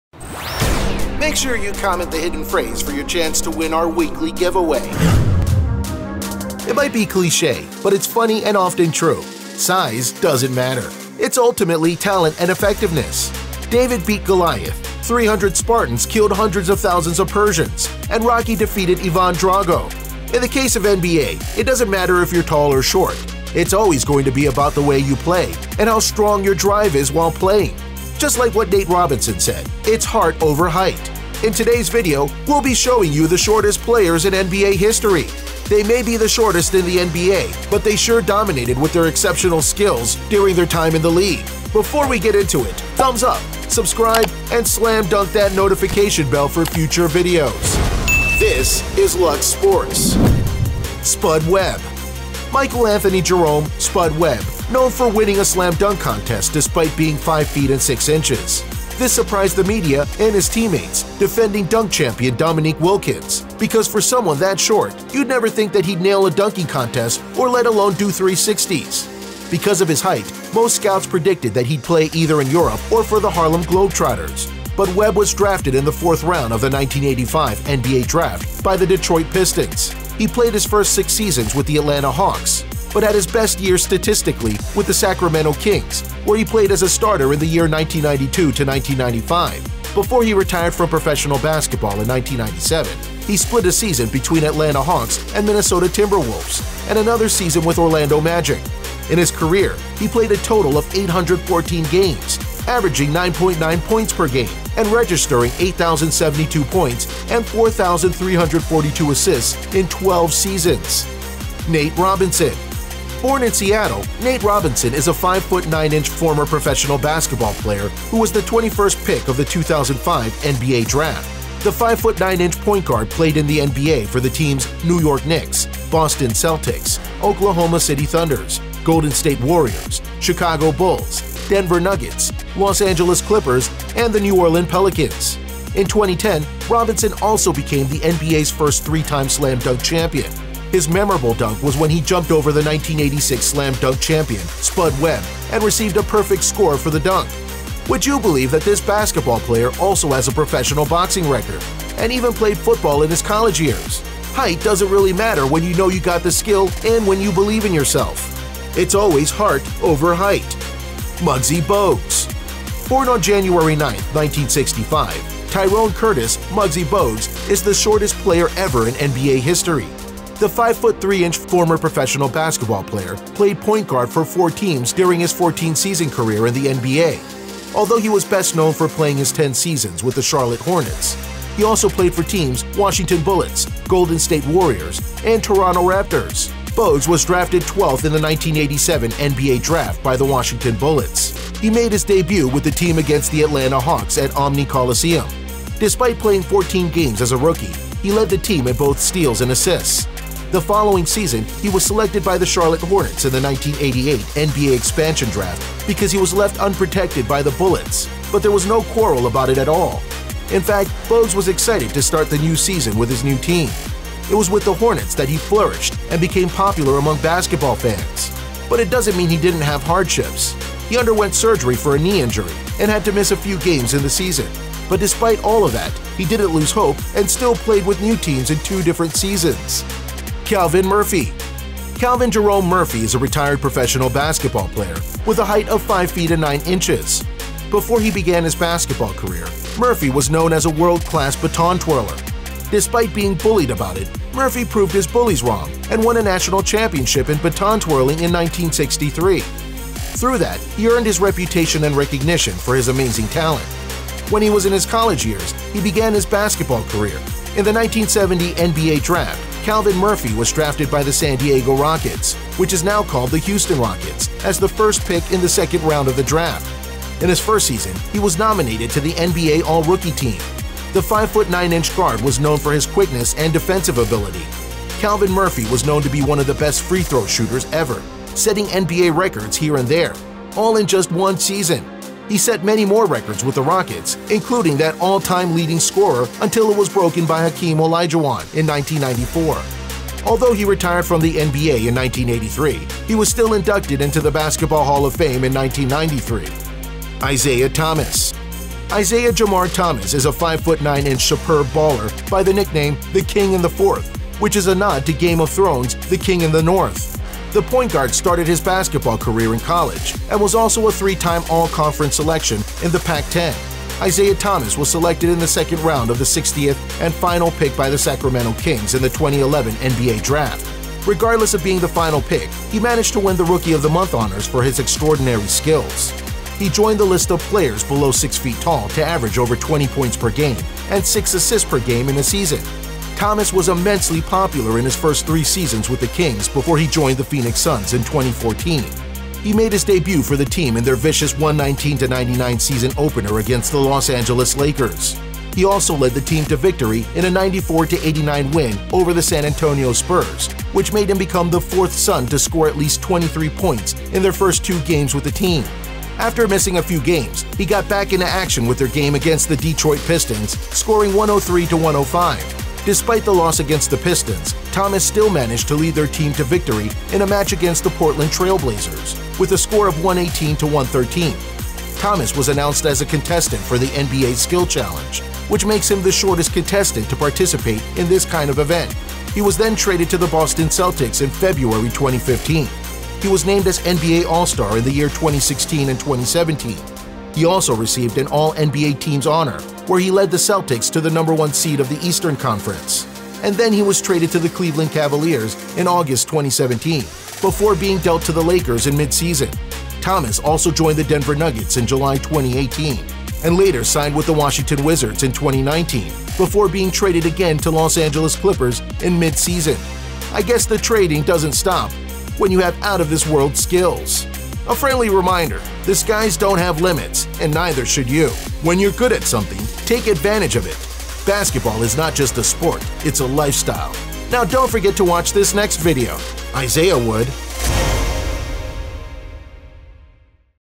Shortest Players in NBA History
Who is the Shortest Players in NBA History? when did the Shortest Players in NBA History play? Join us as we count down the Shortest Players in NBA History?

Top 10 Shortest Players In NBA History

4 Shortest Players In NBA History